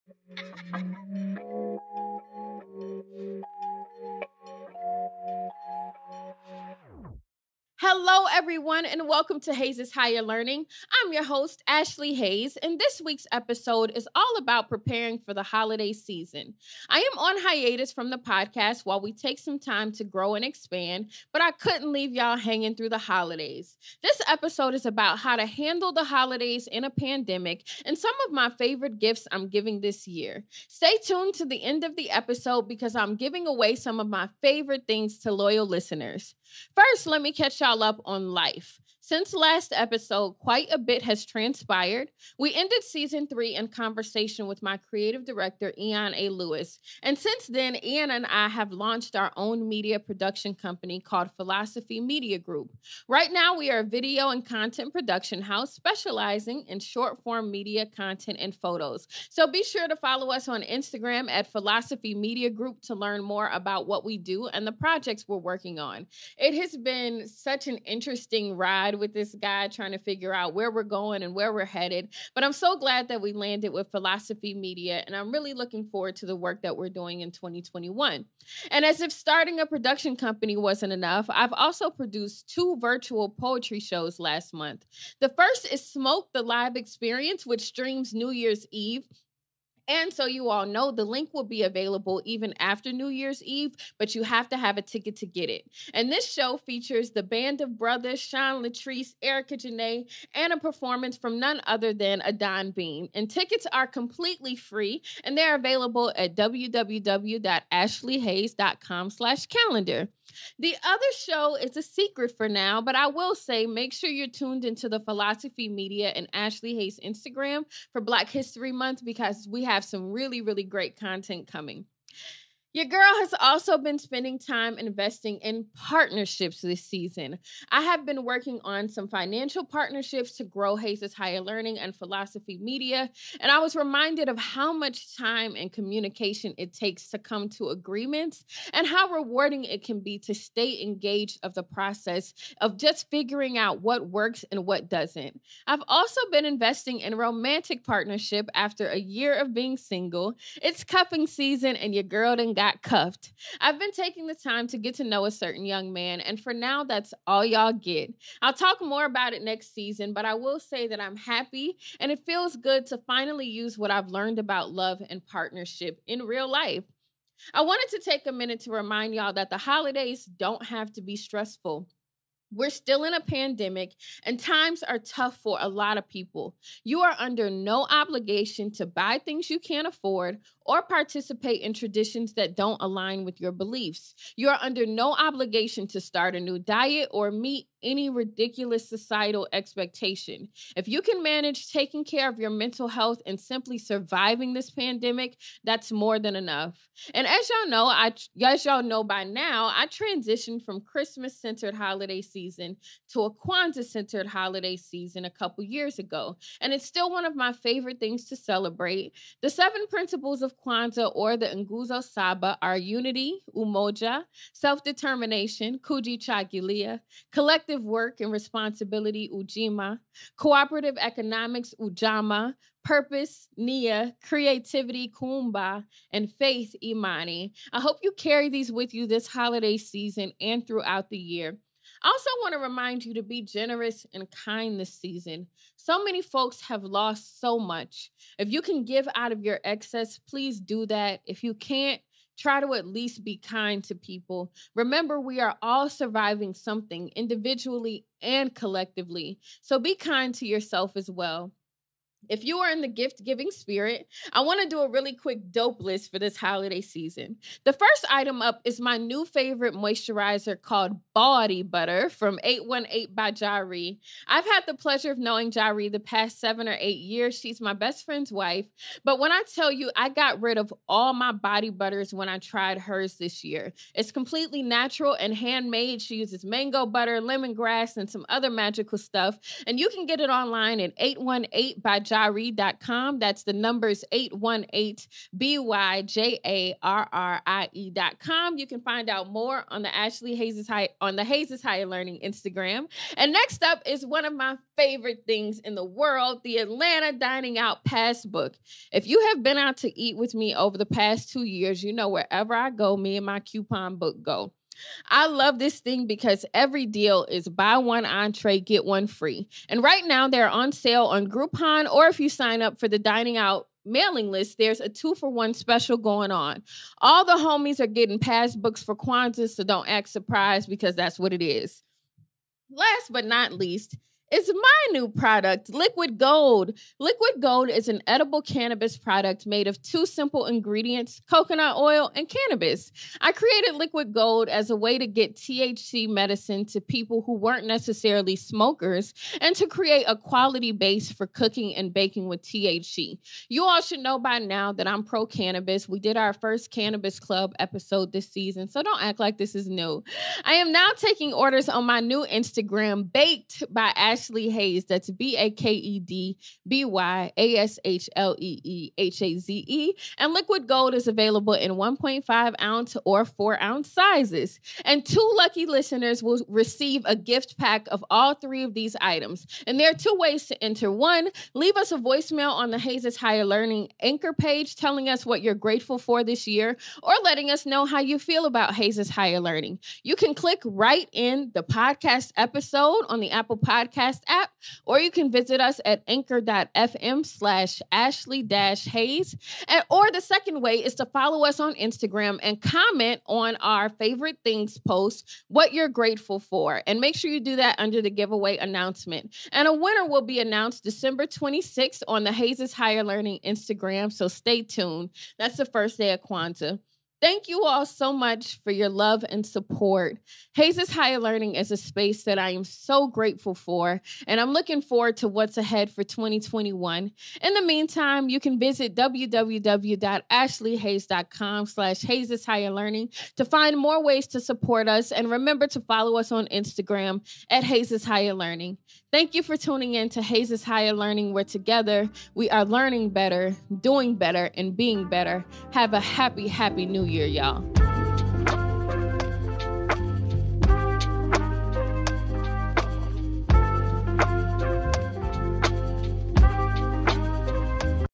Holiday Special: The Dope List
In this Holiday Special, host Ashlee Haze talks about how to cope with the holidays during a pandemic and a few of her favorite things.